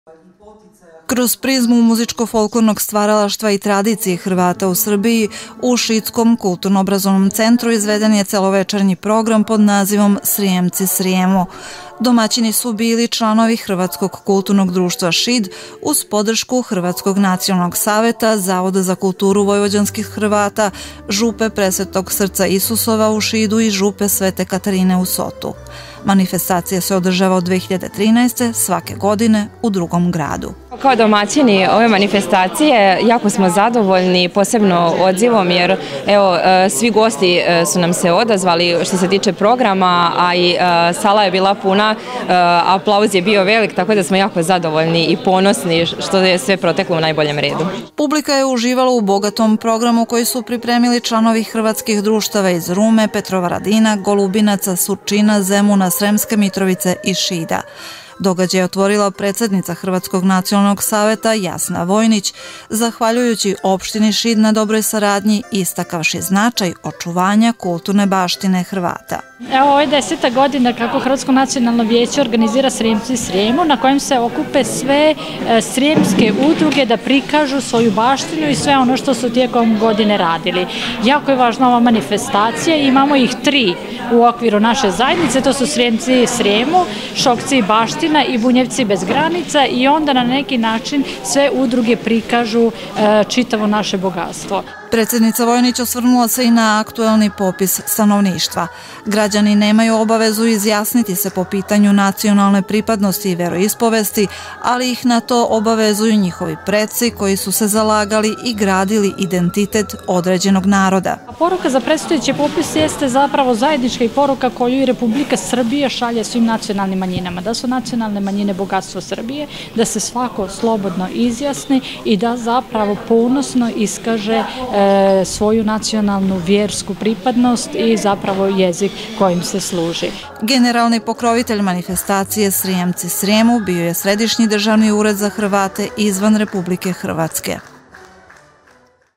Srijemci Srijemu
Deseta manifestacija pod nazivom „Srijemci Srijemu” održana je protekle subote u Šidu. U programu su učestvovala hrvatska društva iz Rume, Petrovaradina, Golubinaca, Surčina, Zemuna, Sremske Mitrovice i Šida. Događaj je otvorila predednica Nacionalnog saveta Hrvata Jasna Vojnić. U programu je prikazana bogata paleta kulturnog nasleđa Hrvata u Srbiji.